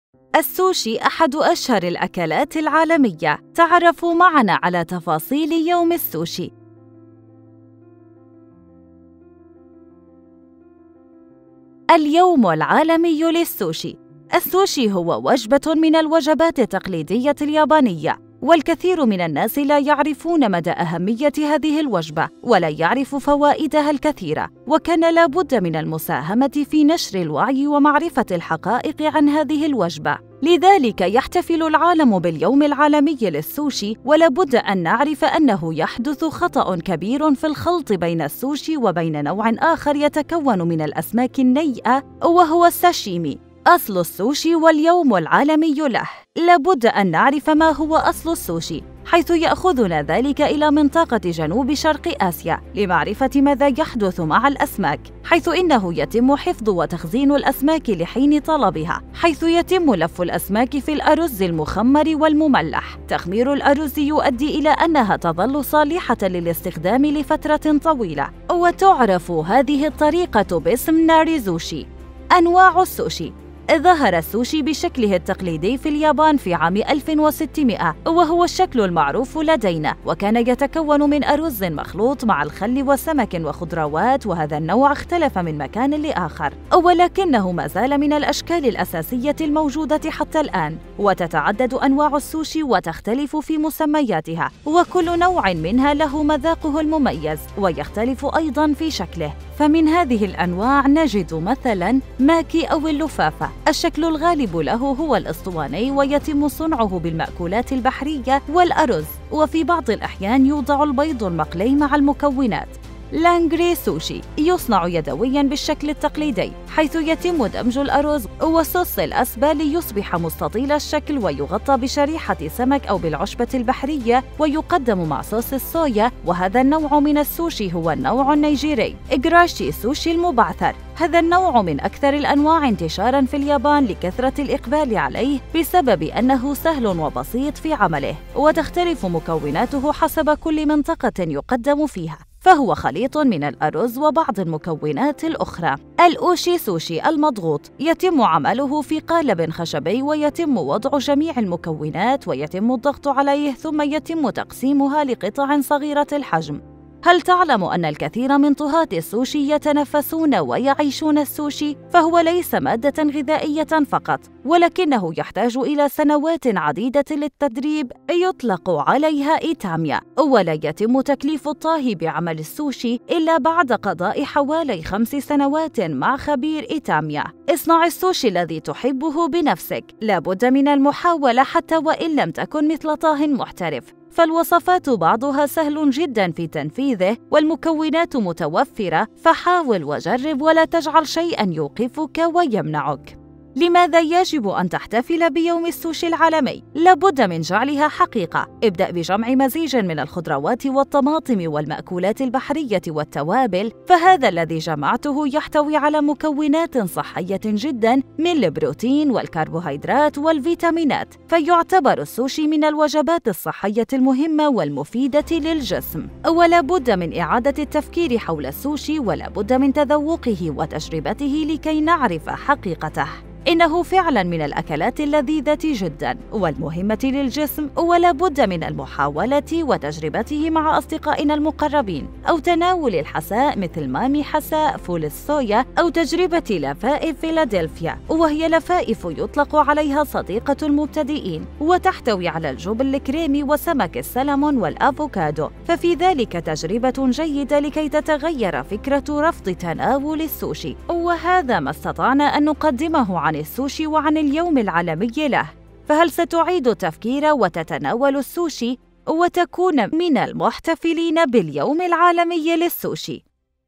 تاريخ يوم السوشي
في حلقة اليوم من برنامج قصة_يوم تعرفوا على تاريخ يوم السوشي العالمي

السوشي أحدُ أشهرِ الأكلاتِ العالميةِ.. تعرفوا معنا على تفاصيلِ يومِ السوشي
اليومُ العالميُّ للسوشي
السوشي هو وجبةٌ من الوجباتِ التقليديةِ اليابانيةِ، والكثيرُ من الناسِ لا يعرفونَ مدى أهميةِ هذهِ الوجبةِ ولا يعرفُ فوائدَها الكثيرةَ، وكانَ لا بدَّ من المساهمةِ في نشرِ الوعيِ ومعرفةِ الحقائقِ عن هذهِ الوجبةِ، لذلك يحتفلُ العالمُ باليومِ العالميِّ للسوشي، ولا بدَّ أن نعرفَ أنهُ يحدثُ خطأٌ كبيرٌ في الخلطِ بينَ السوشي وبينَ نوعٍ آخرَ يتكونُ من الأسماكِ النِّيئَةِ وهوَ الساشيمى.
اصلُ السوشي واليومُ العالميِّ له
لا بدَّ أن نعرفَ ما هو أصلُ السوشي حيثُ يأخذُنا ذلكَ إلي منطقةِ جنوبِ شرقِ آسيا لمعرفةِ ماذا يحدثُ معَ الأسماكِ، حيثُ إنهُ يتمُّ حفظُ وتخزينُ الأسماكِ لحينِ طلبِها؛ حيثُ يتمُّ لفُّ الأسماكِ في الأرزِ المخمَّرِ والمملحِ.
تخميرُ الأرزِ يؤدِّي إلى أنها تظلُّ صالحةً للاستخدامِ لفترةٍ طويلةٍ، وتعرفُ هذهِ الطريقةُ باسمِ Narezushi.

أنواعُ السوشي
ظهرَ السوشي بشكلِهِ التقليديِّ في اليابانِ في الفترةِ ما بينَ ألفٍ وستِّ مئةٍ وحتي ألفٍ وستِّ مئةٍ وهوَ الشكلُ المعروفُ لدينا، وكانَ يتكونُ من أرزٍ مخلوطٍ معَ الخلِّ وسمكٍ وخضراواتٍ وهذا النوعُ اختلفَ من مكانٍ لآخرَ، ولكنهُ ما زالَ من الأشكالِ الأساسيةِ الموجودةِ حتي الآن.
وتتعددُ أنواعُ السوشي وتختلفُ في مسمياتِها.
وكلُّ نوعٍ منها له مذاقُهُ المميزُ، ويختلفُ أيضًا فى شكلِهِ،
فمنْ هذهِ الأنواعِ نجدُ مثلًا:
ماكي أو اللفافةُ، الشكلُ الغالبُ له هو الأسطوانيُّ ويتمُّ صنعُهُ بالمأكولاتِ البحريةِ والأرزِ وفي بعضِ الأحيانِ يوضعُ البيضُ المقليُّ معَ المكوناتِ.
لينغرى سوشي يصنعُ يدويًّا بالشكلِ التقليديِّ؛ حيثُ يتمُّ دمجُ الأرزِ وصوصِ الأسبى ليصبحَ مستطيلَ الشكلِ ويغطَّي بشريحةِ سمكٍ أو بالعشبةِ البحريةِ ويقدَّمُ مع صوص الصويا، وهذا النوعُ منَ السوشي هو النوعُ النيجيريُّ.
جراشي سوشي المبعثرُ، هذا النوعُ من أكثرِ الأنواعِ انتشارًا في اليابانِ لكثرةِ الإقبالِ عليهِ بسببِ أنه سهلٌ وبسيطٌ في عملِهِ وتختلفُ مكوناتُهُ حسبَ كلِّ منطقةٍ يقدمُ فيها، فهوَ خليطٌ من الأرزِ وبعضِ المكوناتِ الأخرى.
الأوشي سوشي المضغوطُ يتمُّ عملُهُ في قالبٍ خشبيٍّ ويتمُّ وضعُ جميعِ المكوناتِ ويتمُّ الضغطُ عليها ثم يتمُّ تقسيمُها لقطعٍ صغيرةِ الحجمِ.
هل تعلمُ أنَّ الكثيرَ من طهاةِ السوشي يتنفسونَ ويعيشونَ السوشي، فهو ليسَ مادةً غذائيةً فقطْ ولكنهُ يحتاجُ إلى سنواتٍ عديدةٍ للتدريبِ يطلقُ عليها itamea ولا يتمُّ تكليفُ الطاهي بعملِ السوشي، إلا بعدَ قضاءِ حوالي خمسِ سنواتٍ مع خبيرٍ itamea.
اصنعِ السوشي الذى تحبُّهُ بنفسِكَ
لا بدَّ منَ المحاولةِ حتي وإن لم تكنْ مثلَ طاهٍ محترفٍ، فالوصفاتُ بعضُها سهلٌ جدًّا في تنفيذِهِ والمكوناتُ متوفرةٌ فحاولْ وجربْ ولا تجعلْ شيئًا يوقفُكَ ويمنعُكَ.
لماذا يجبُ أن تحتفلَ بيومِ السوشي العالميِّ
لا بدَّ من جعلِها حقيقةً.. ابدأ بجمعِ مزيجٍ من الخضراواتِ والطماطمِ والمأكولاتِ البحريةِ والتوابلِ، فهذا الذي جمعتَهُ يحتوي على مكوناتٍ صحيةٍ جدًّا من البروتينِ والكربوهيدراتِ والفيتاميناتِ فيعتبرُ السوشي من الوجباتِ الصحيةِ المهمةِ والمفيدةِ للجسمِ
ولا بدَّ من إعادةِ التفكيرِ حولَ السوشي ولا بدَّ من تذوقِهِ وتجربتِه لكي نعرفَ حقيقتَهُ.. إنهُ فعلًا منَ الأكلاتِ اللذيذةِ جدًّا والمهمةِ للجسمِ ولا بدَّ منَ المحاولةِ وتجربتِهِ مع أصدقائِنا المقربينَ أو تناولِ الحساءَ مثلَ مامي حساء فول الصويا أو تجربةِ لفائفِ philadaphia وهي لفائفُ يطلقُ عليها صديقةُ المبتدئينَ وتحتوي علي الجبنِ الكريمى وسمكِ السلمون والأفوكادو، ففي ذلكَ تجربةٌ جيدةٌ لكي تتغيرْ فكرةُ رفضِ تناولِ السوشي.
وهذا ما استطعنا أن نقدمَهُ عن السوشي وعن اليومِ العالميِّ له، فهل ستعيدُ التفكيرَ وتتناولُ السوشي وتكونُ من المحتفلينَ باليومِ العالميِّ للسوشي.